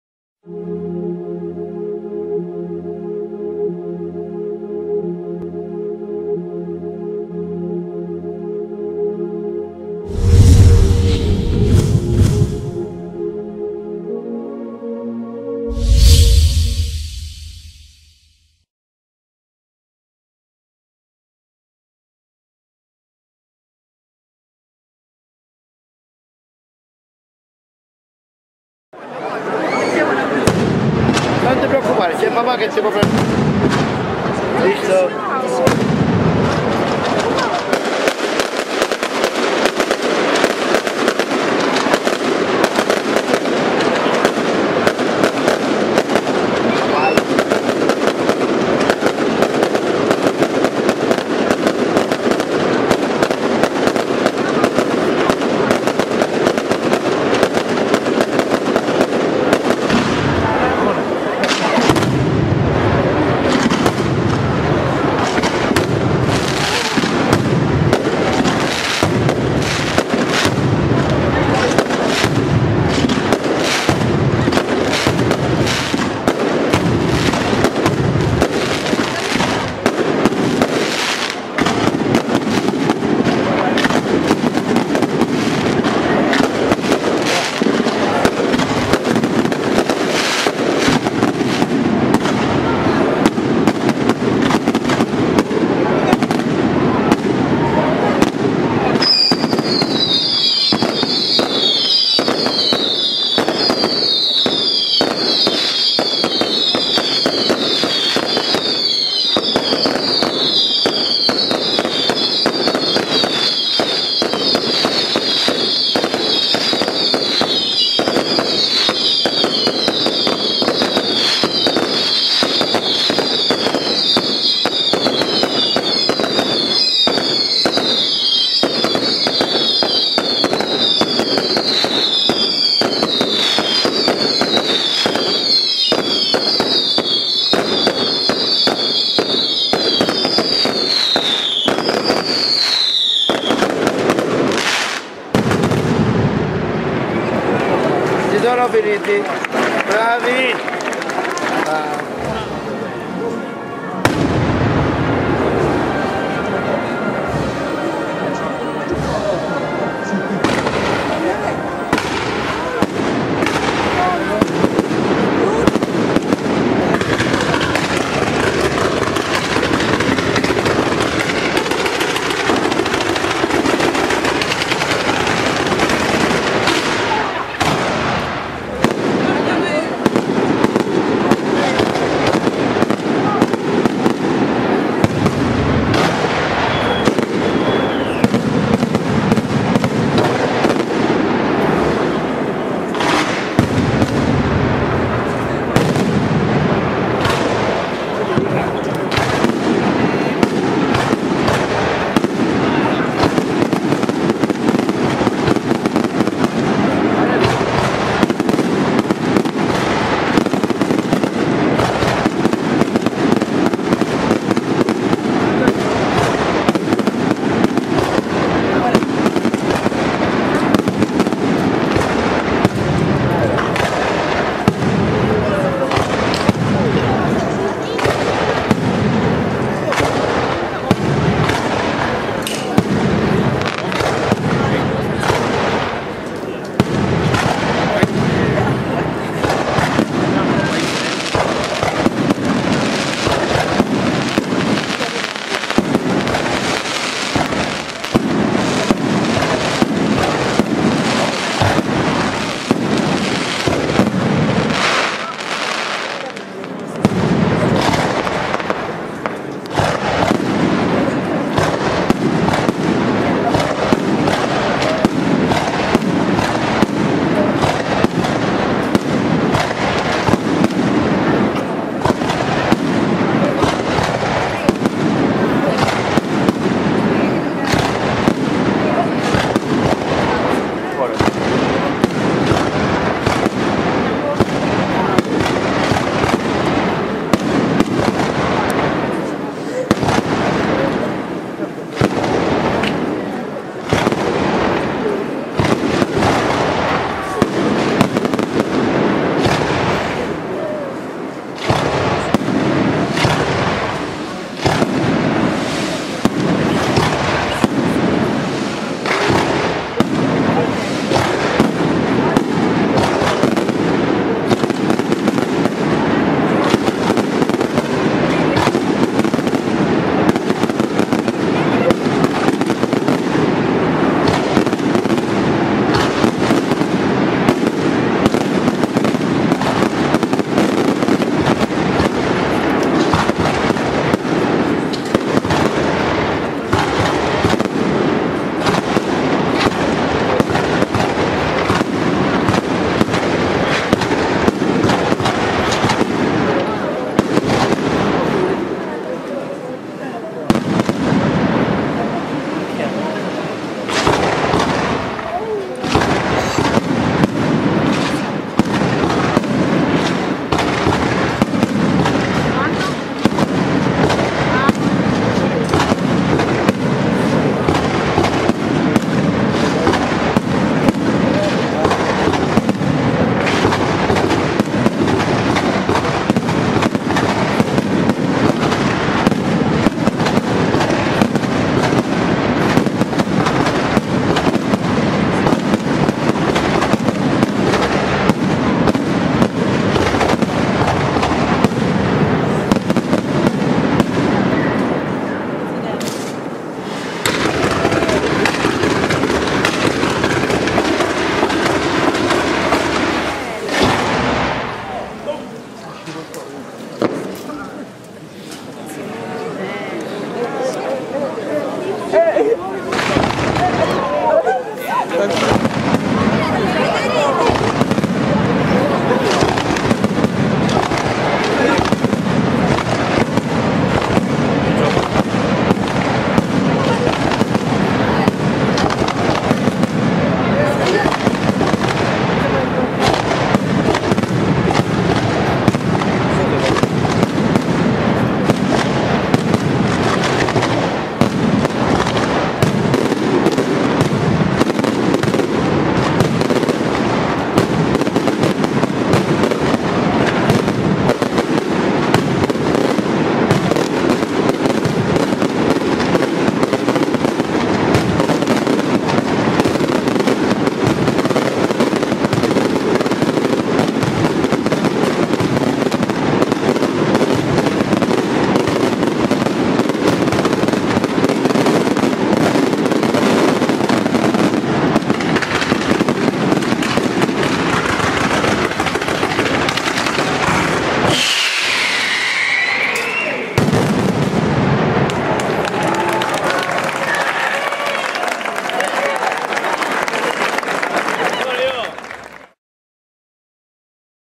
ISPICA (Rg) - GIOVEDI' SANTO 2018 - PIROTECNICA IBLEA
Ispica (Rg) 29 Marzo 2018
"Giovedì Santo"

Fuochi Pirotecnici della
PIROTECNICA IBLEA di Lorenzo Massari da Ragusa
Riprese Video by Simone Vinciguerra